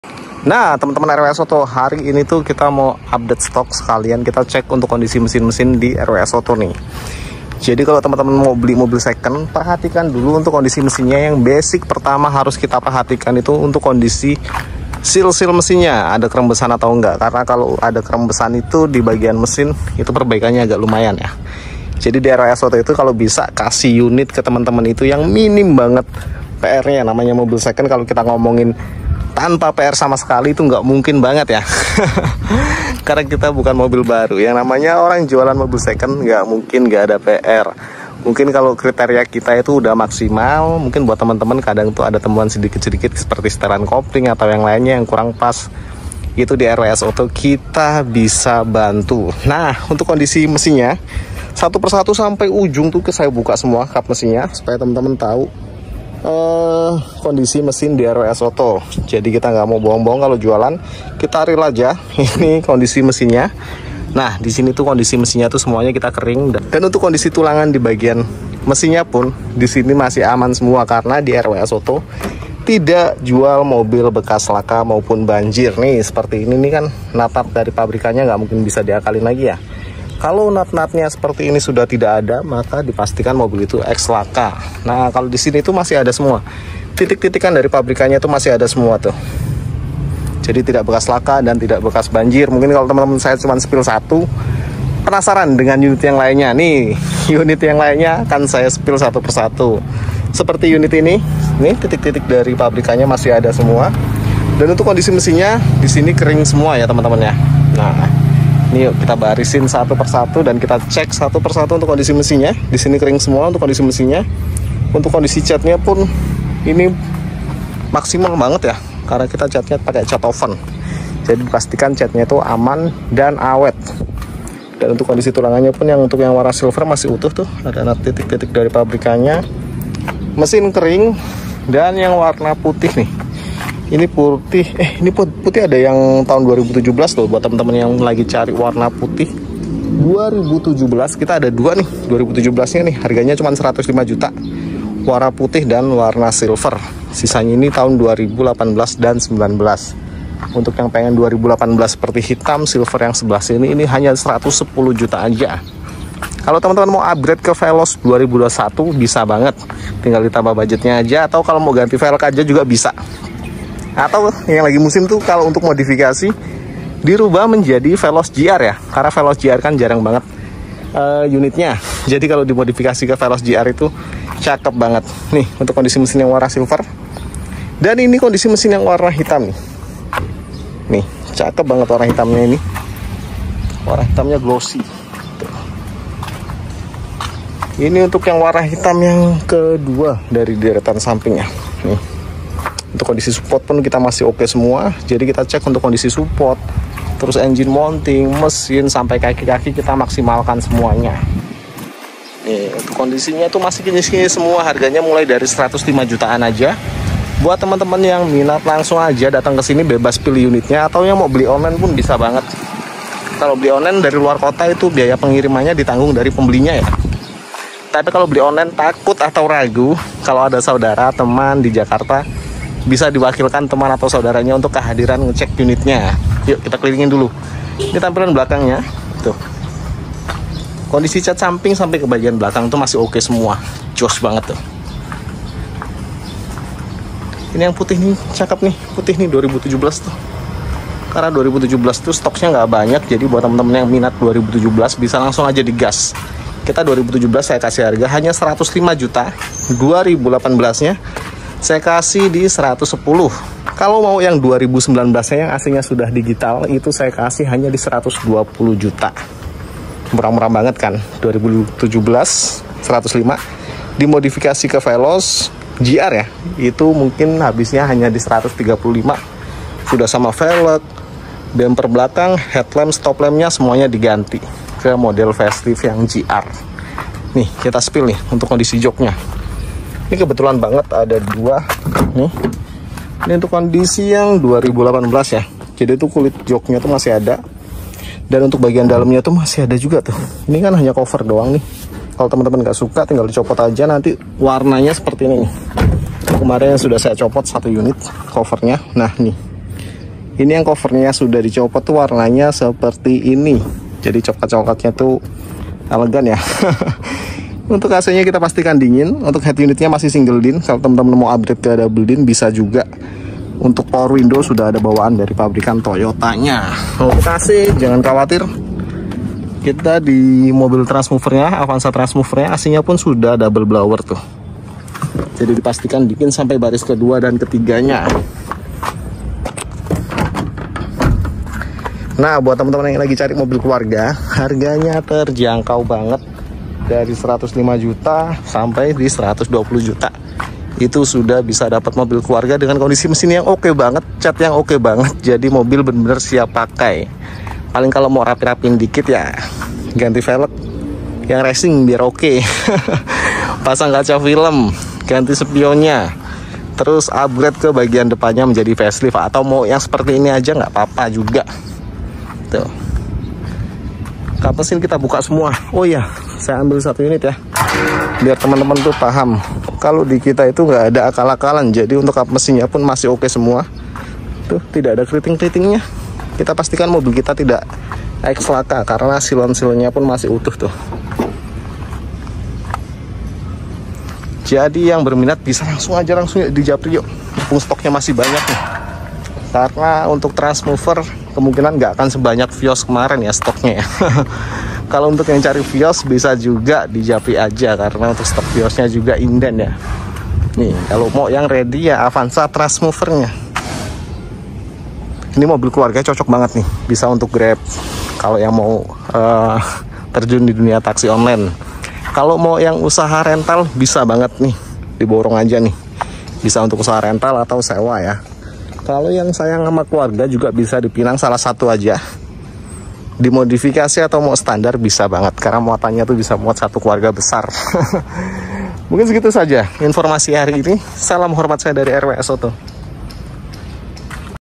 Jangan sampai beli mobil bekas laka atau oli mesin rembes || BAHAYA!!!
Wajib tonton video ini sebelum beli mobil bekas taksi di tempat lain